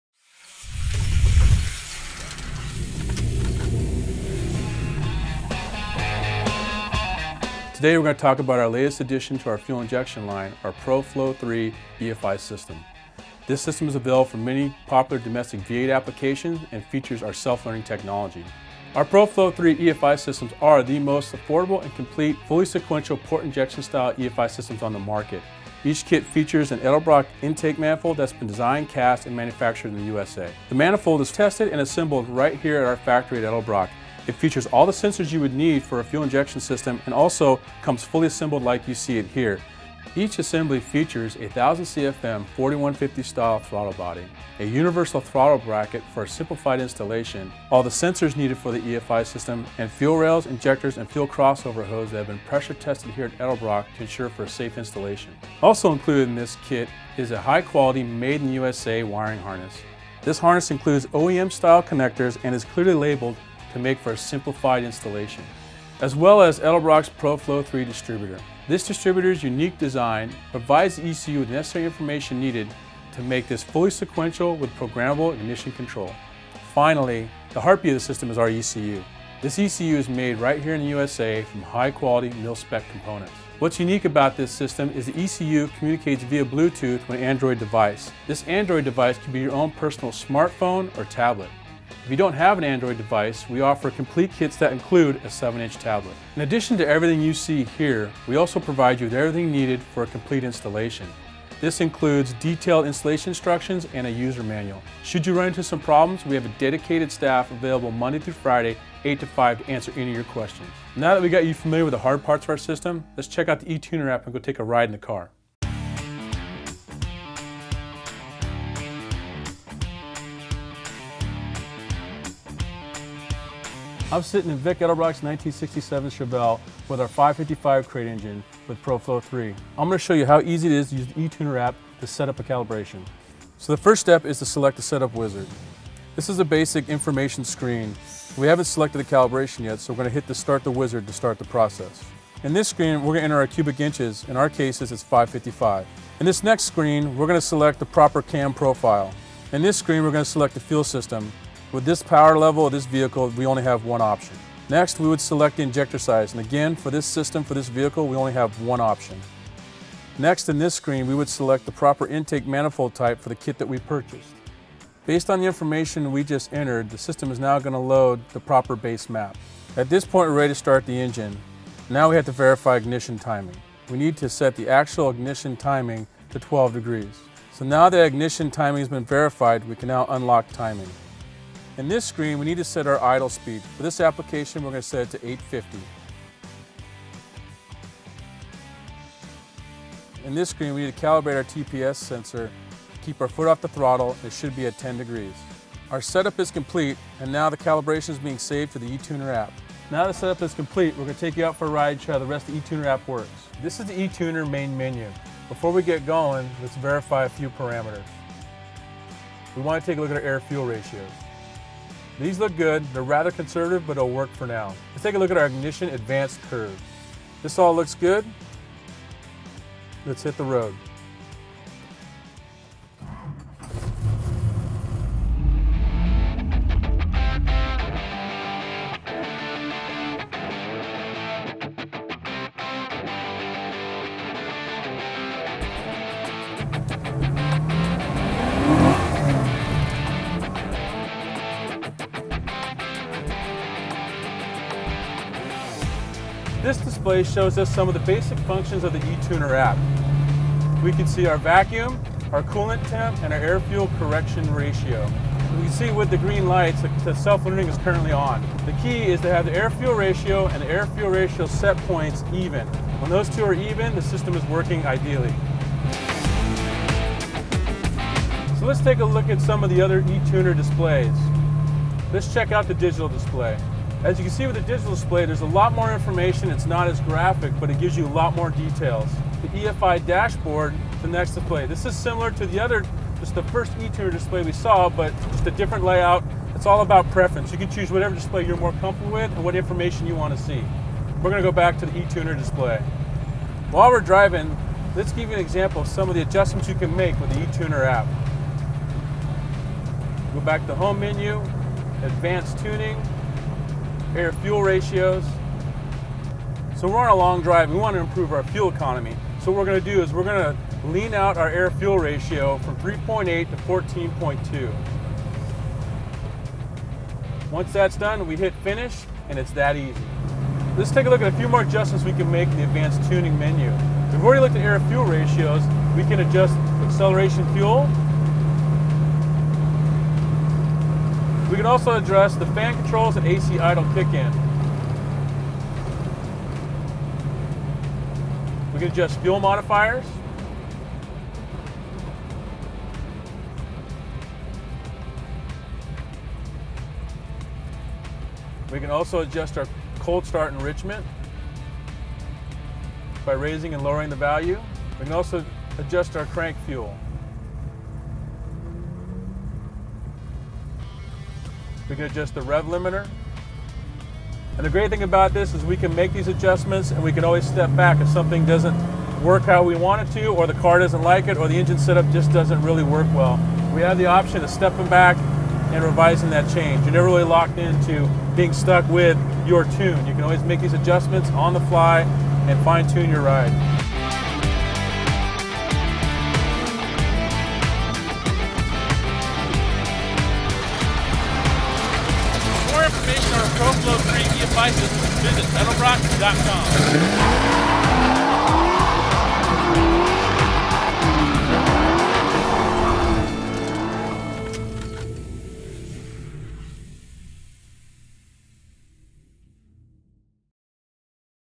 Pro-Flo 3 EFI System
Pro-Flo 3 is a step up from our popular entry level E-Street throttle-body EFI system. Pro-Flo 3 offers the ultimate in performance and power with the precise efficiency only a fully sequential port EFI system can deliver. How does a sequential port EFI system differ from a throttle-body style EFI system? A fully sequential port EFI system delivers the fuel into the intake air flow right at the port with an injector for each cylinder, allowing for better fuel atomization and distribution.